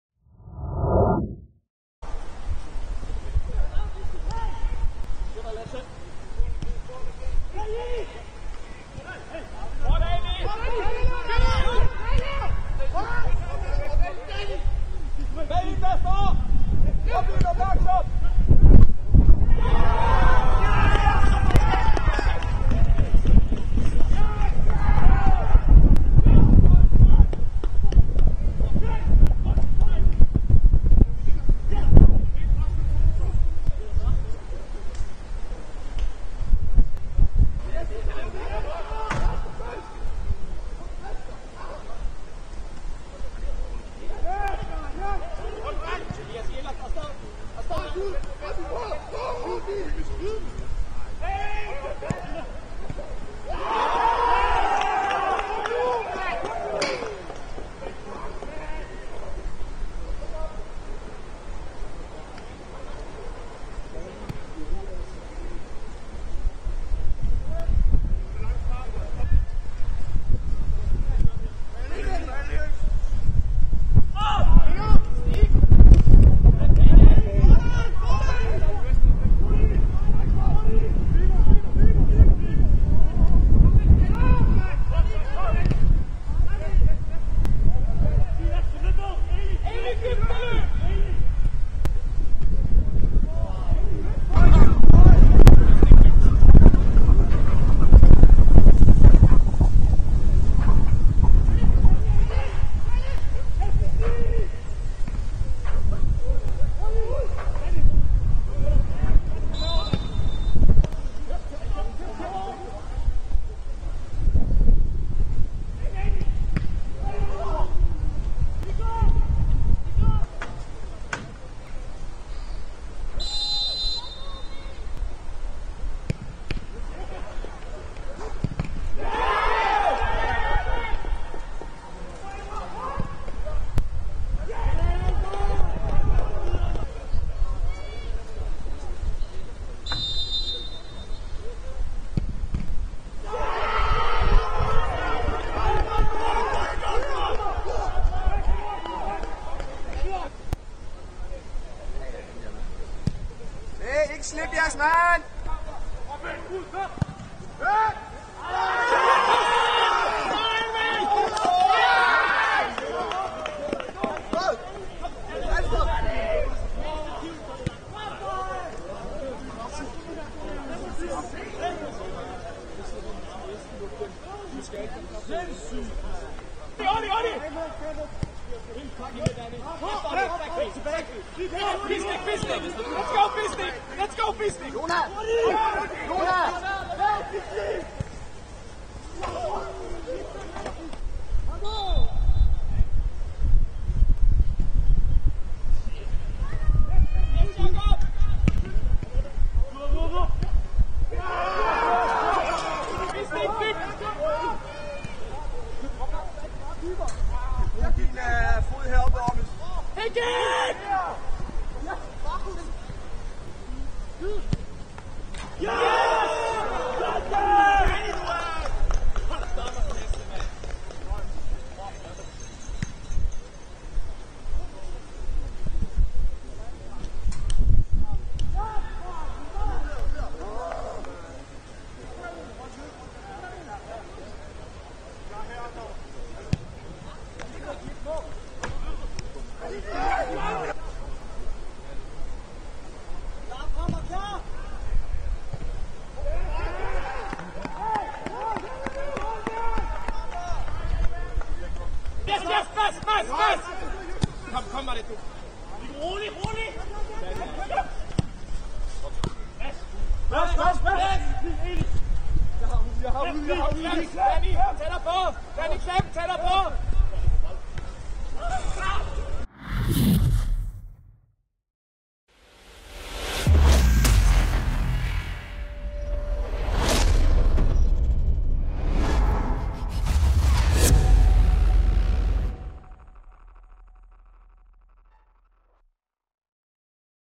U19-Highlights: OB 2-4 FCK
U19 kan gå på vinterferie med en 4-2-sejr i Odense i weekenden, hvor Orri Oskarsson var på pletten to gange og keeper Albert Gaub reddede to straffespark.

Målscorere var som følger:
0-1: Orri Oskarsson
0-2: Selvmål
0-3: Orri Oskarsson
1-3: OB
1-4: Daniel Haarbo
2-4: OB

profilnavn: fc_kobenhavn